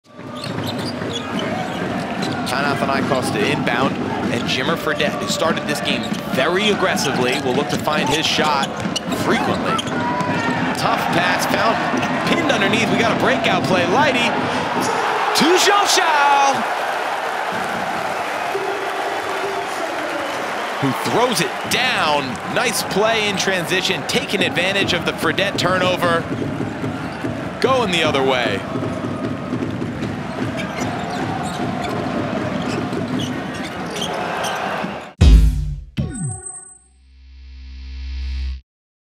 7DAYS Magic Moment of the Night: LDLC ASVEL Villerubanne
ASVEL runs a nice fast break that Livio Jean-Charles finishes with a big slam.